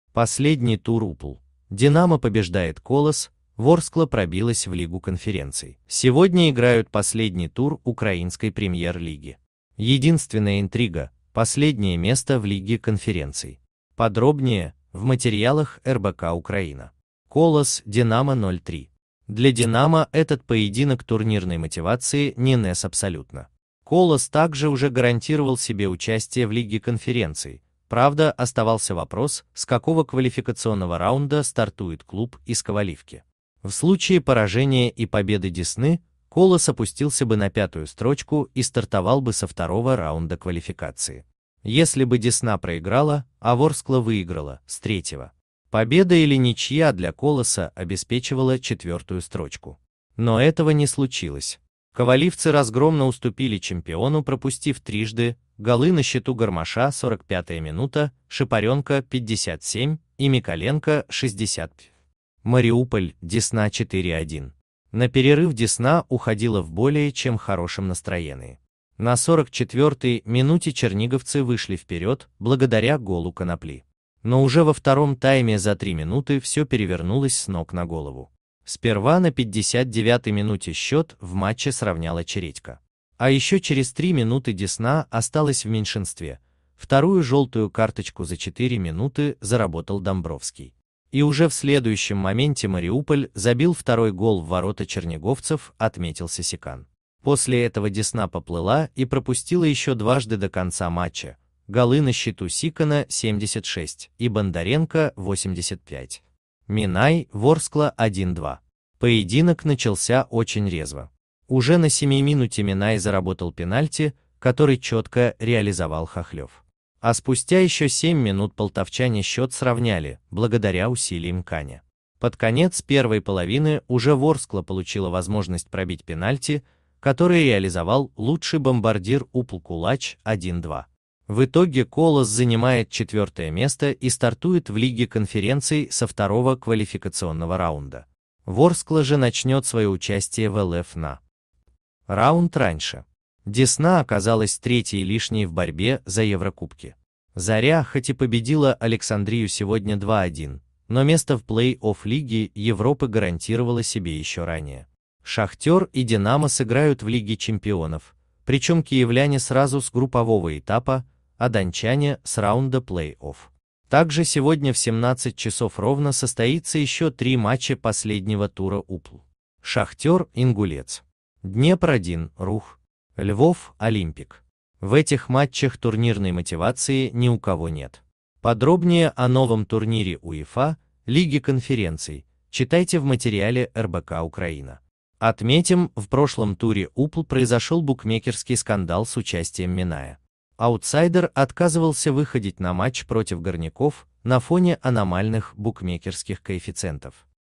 Последний тур УПЛ: "Динамо" побеждает "Колос", "Ворскла" пробилась в Лигу конференций.
Новости Спорт Ворскла Динамо УПЛ Сегодня играют последний тур Украинской Премьер-лиги. Единственная интрига - последнее место в Лиге конференций. Подробнее - в материалах РБК-Украина. "Колос" - "Динамо" 0:3.
Для "Динамо" этот поединок турнирной мотивации не нес абсолютно.  "Колос" также уже гарантировал себе участие в Лиге конференций, правда, оставался вопрос, с какого квалификационного раунда стартует клуб из Коваливки.  В случае поражения и победы "Десны", "Колос" опустился бы на пятую строчку и стартовал бы со 2-го раунда квали